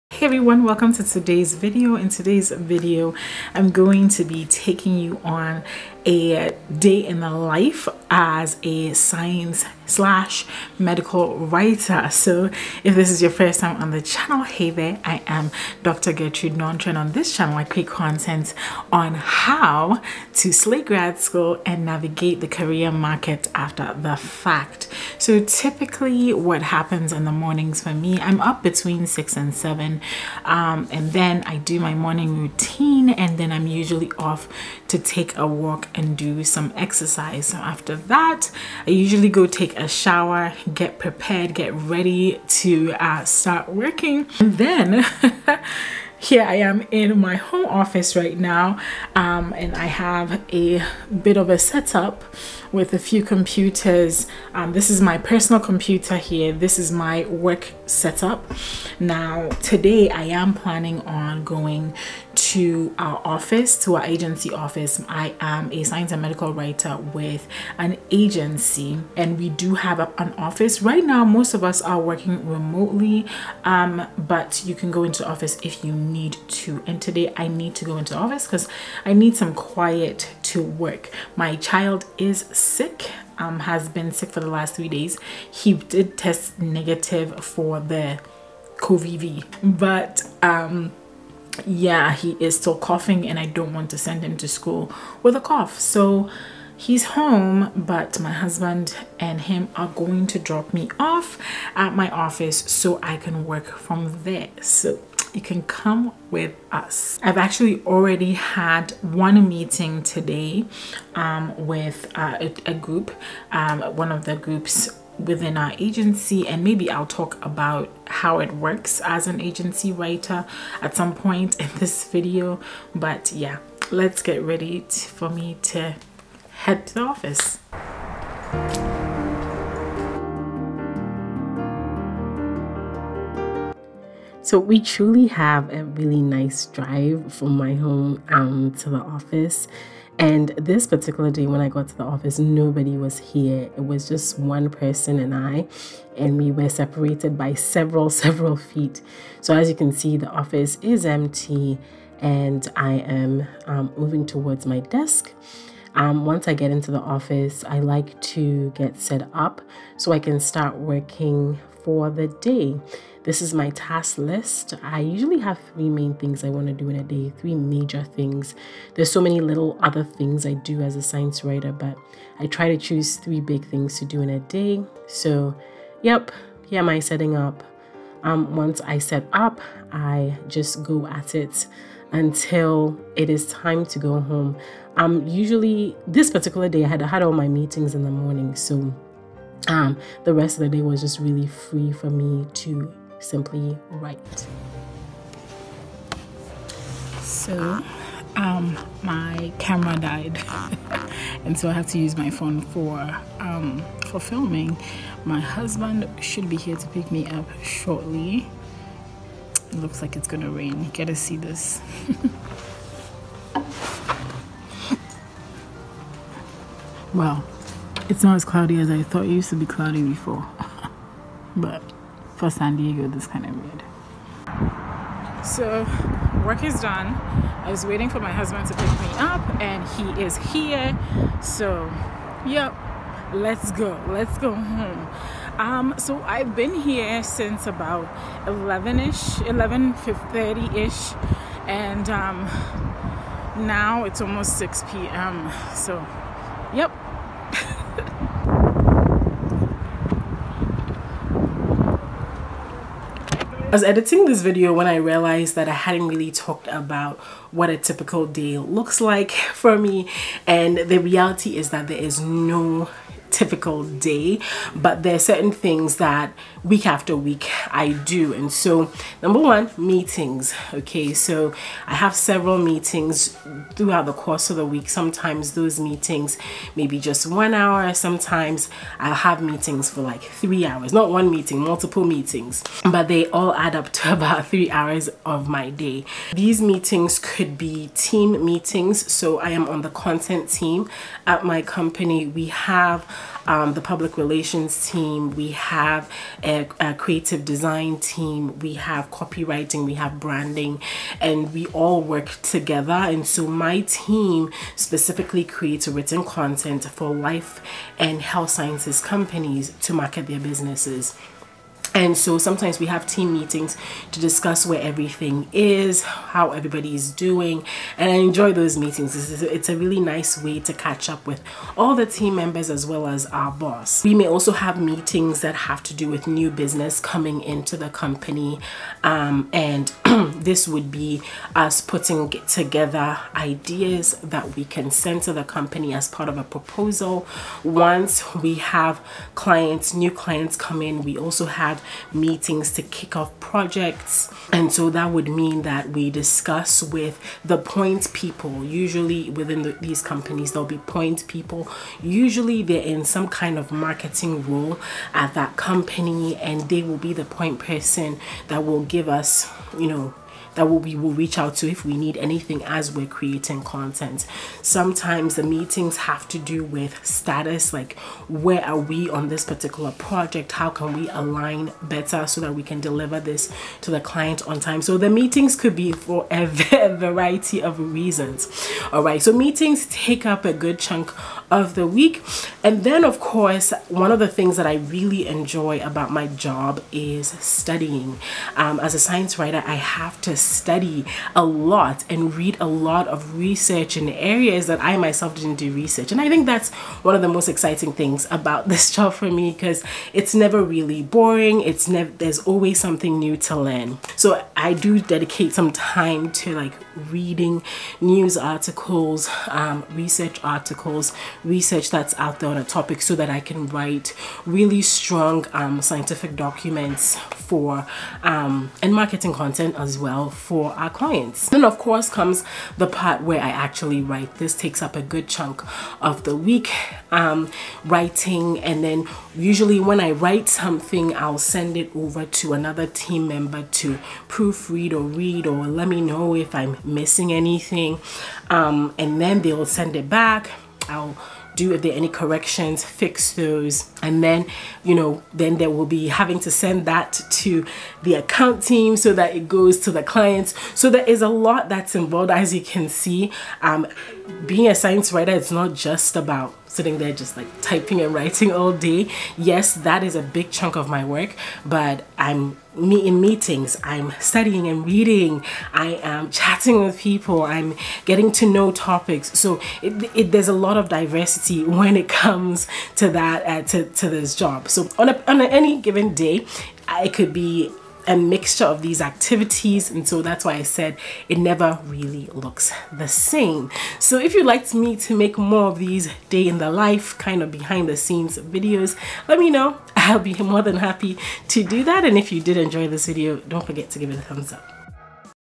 What do MEDICAL WRITERS do all day? Day in the life of a medical writer.
What do I do ALL DAY as a MEDICAL WRITER? Day in the life vlog. Hey there PhD friend! In today's video, we're talking about my day as a science and medical writer with a life and health sciences agency.